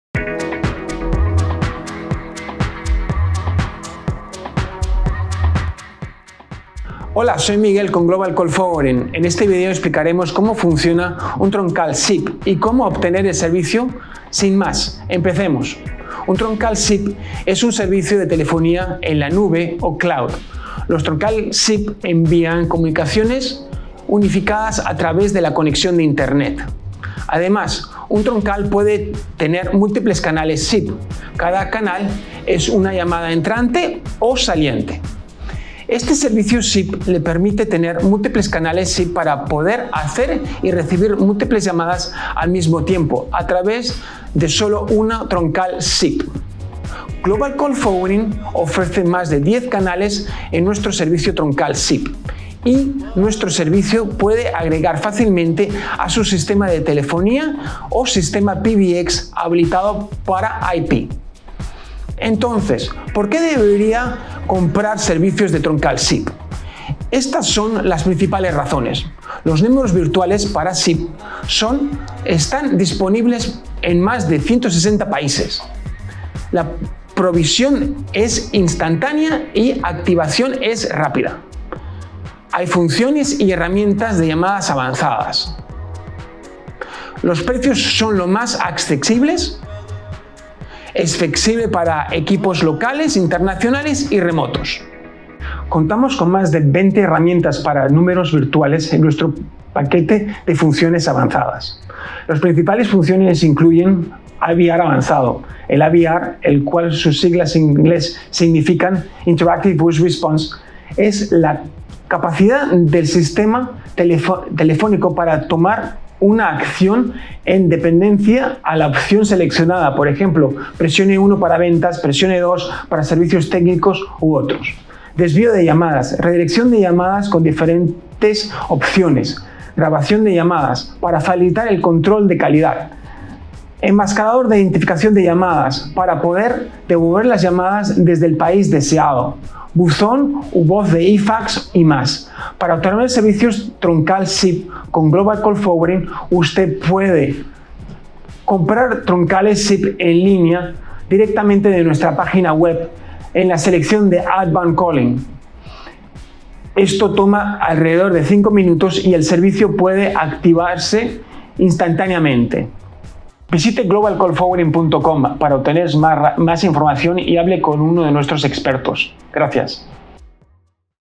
¿Qué es una Troncal SIP?
En este video, explicaremos cómo funciona una Troncal SIP y cómo obtener el servicio.

Una Troncal SIP es un servicio de telefonía en la nube o cloud. Los troncales SIP envían comunicaciones unificadas a través de la conexión de internet.

Una troncal puede tener múltiples canales SIP. Cada canal es una llamada entrante o saliente.

Este servicio SIP le permite tener múltiples canales para poder hacer y recibir múltiples llamadas al mismo tiempo a través de solo una troncal SIP.

Entonces, ¿por qué debería comprar servicios de troncal SIP? Estas son las principales razones:

Los números virtuales para SIP están disponibles en más de 160 países.
La provisión es instantánea y la activación es rápida.
Hay funciones y herramientas de llamada avanzadas.
Los  precios son lo bien asequibles.
Es flexible para equipos locales, internacionales y remotos.

Compre troncales SIP en línea, directamente en nuestra página web en la sección de Outbound Calling.

Visite globalcallforwarding.com para obtener más información y hable con uno de nuestros expertos.

TIMESTAMPS
Introducción 00:00
¿Qué es la Troncalización SIP? 00:16
Beneficios 01:02
Funciones y Servicios 01:35
¿Cómo Comprar Troncales SIP? 02:23